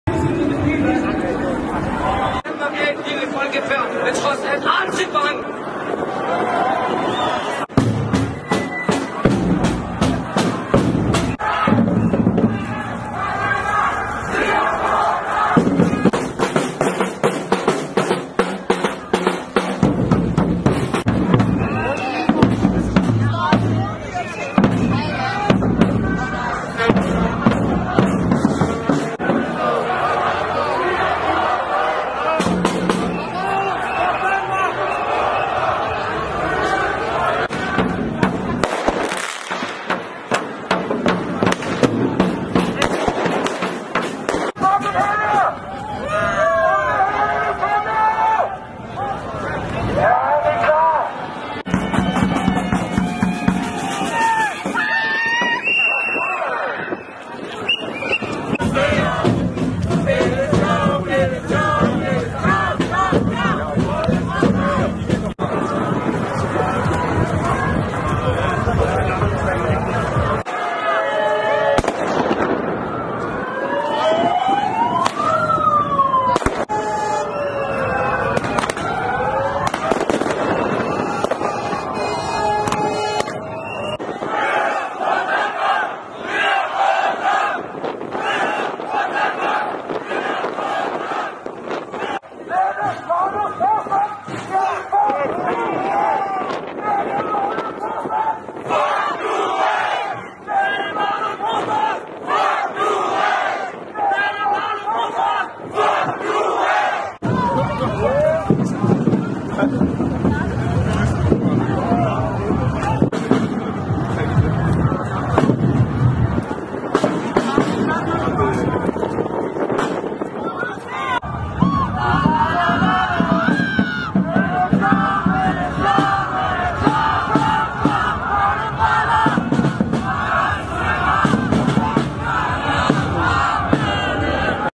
MEN IN BLACK DEMONSTRATION I KØBENHAVN: ‘FRIHEDSDEMONSTRATION’
Søndag eftermiddag og aften demonstreres der i København.
Som mange gange før er demonstrationen arrangeret af Men In Black.